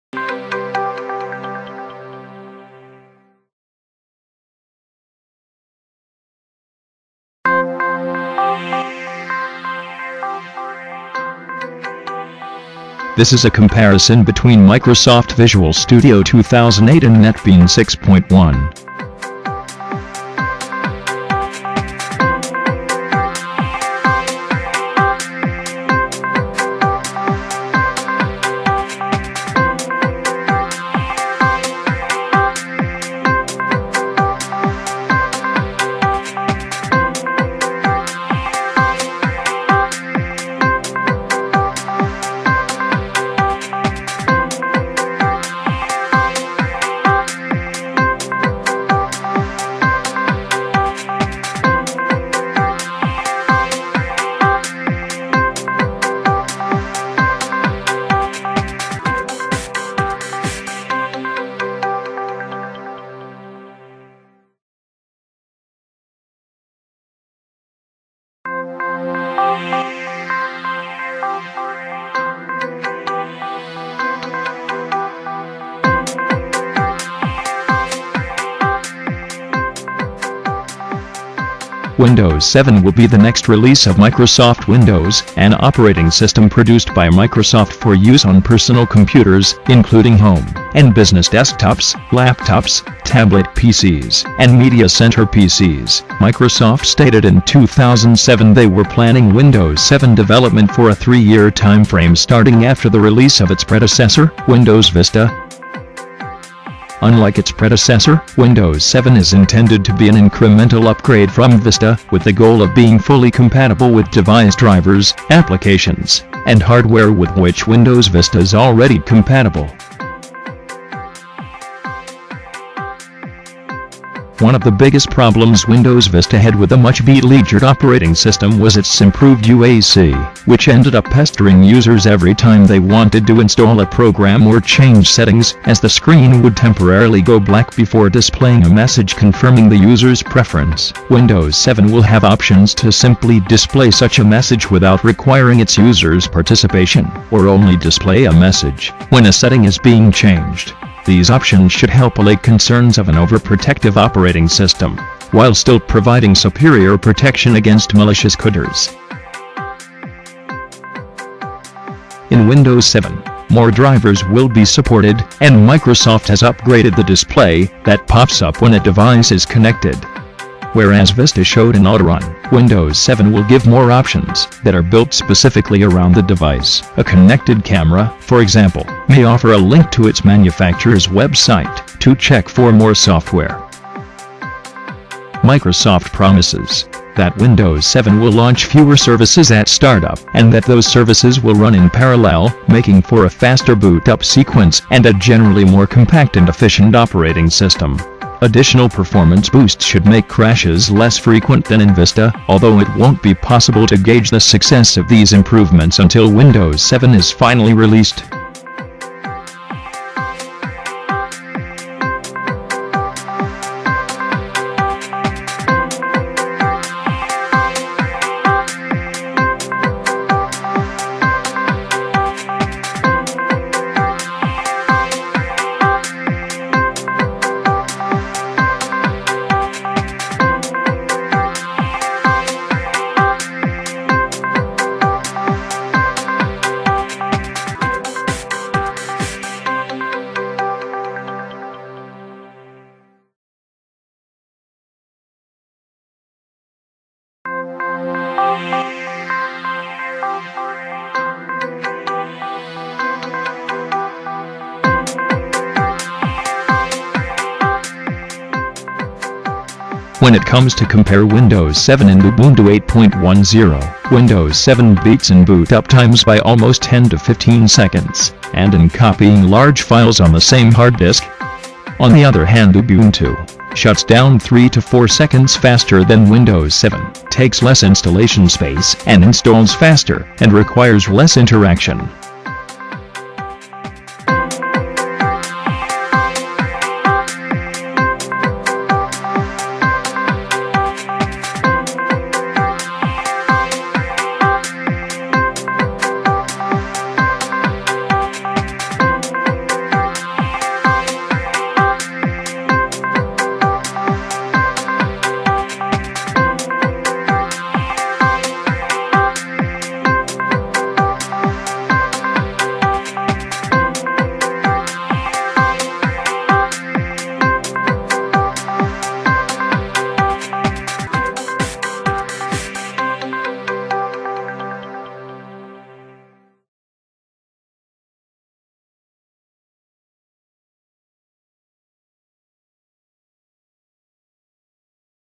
PLDC - New Technologies And Concepts Introduced by Microsoft Corporation (Part 02)
This Video is based on the new technologies and concepts introduced by Microsoft Corporation

Video was created by -

   1. DIT/07/C2/0357  G.W.V. Kanishka
   2. DIT/07/C1/0127  A.A.L. Chamara
   3. DIT/07/C1/0028  S.S.L. Marasinghe
   4. DIT/07/C1/0027  W.C.A Sumathiratne
   5. DIT/07/C1/0031  A.S.S. Liyanaarachchi
   6. DIT/07/C1/0015  M.K. Suzan Deen

References & Materials we used to create this video -

   1. Youtube
   2. AT&T Text to Speech Voice Engine
   3. Nokia Background Sounds
   4. Wikipedia.com
   5. About.com
   6. Microsoft.com